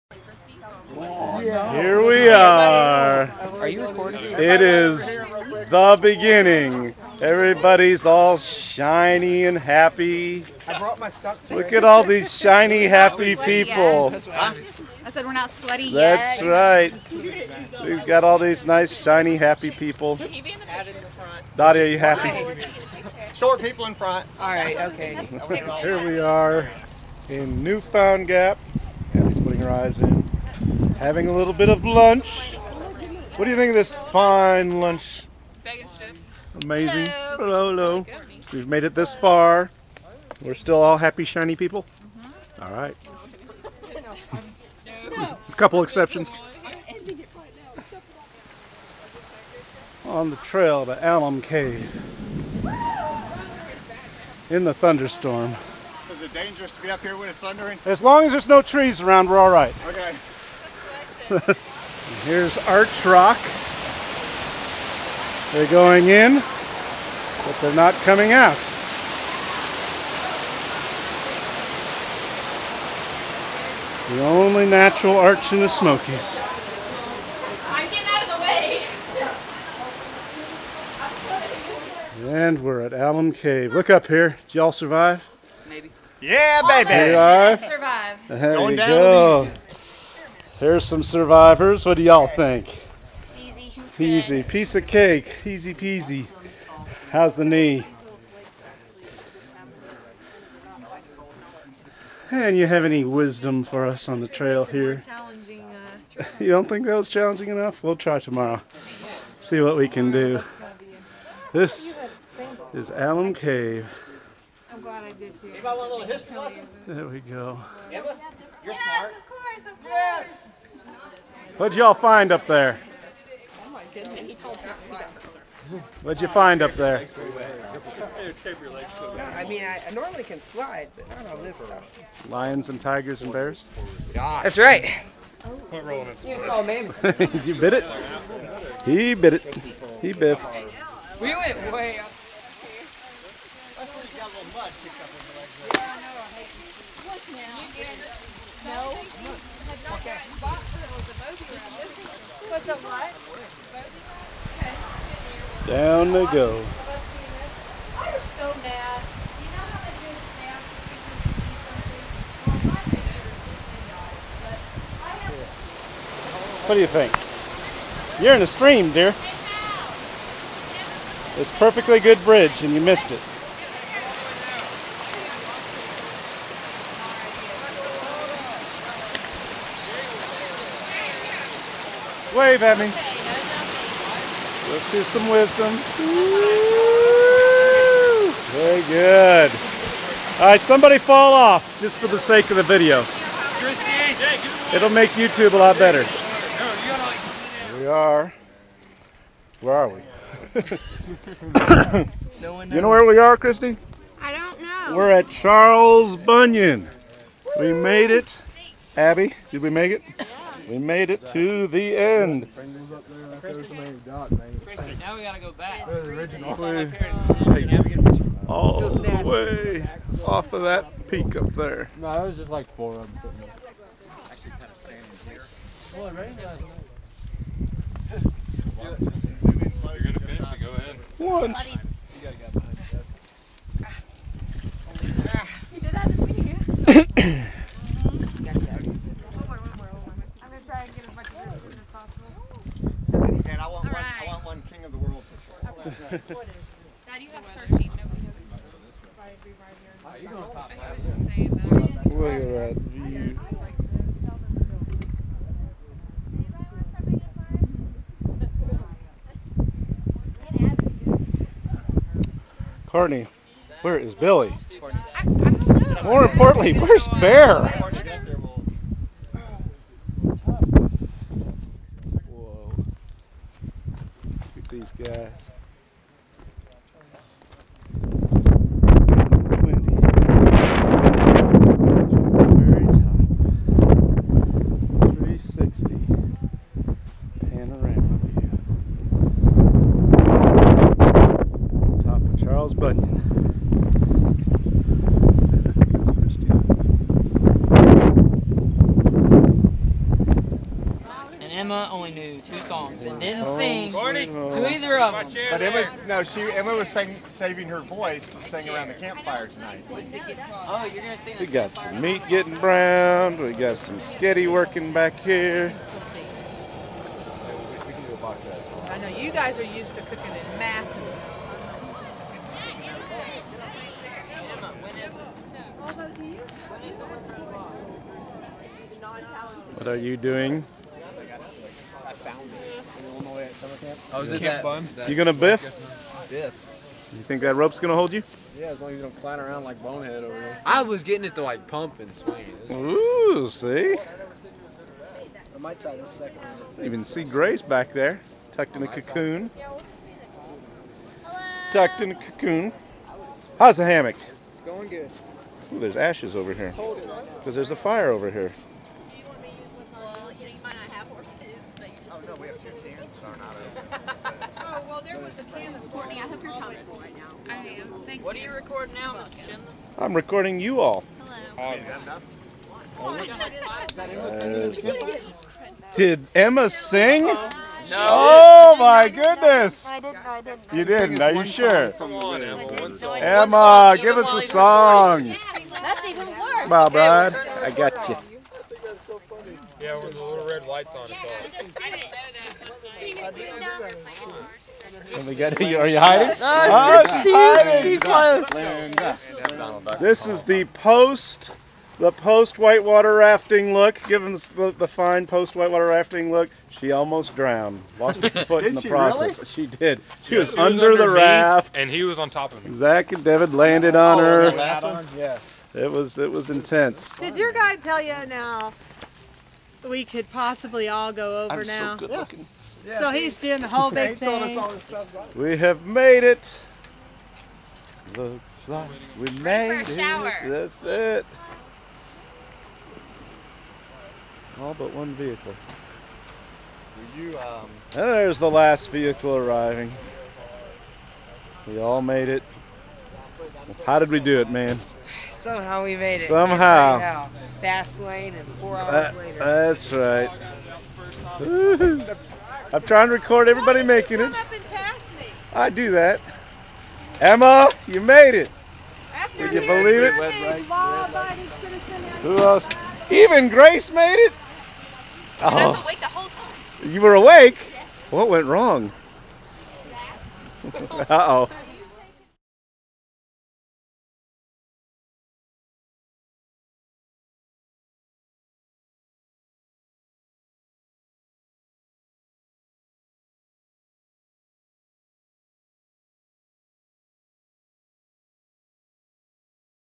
Smokey Mountain Spring Break
First Baptist Church of Conyers 10 grade Sunday School (class of 2014) head to the mountains for camping, hiking, white water rafting, and fun!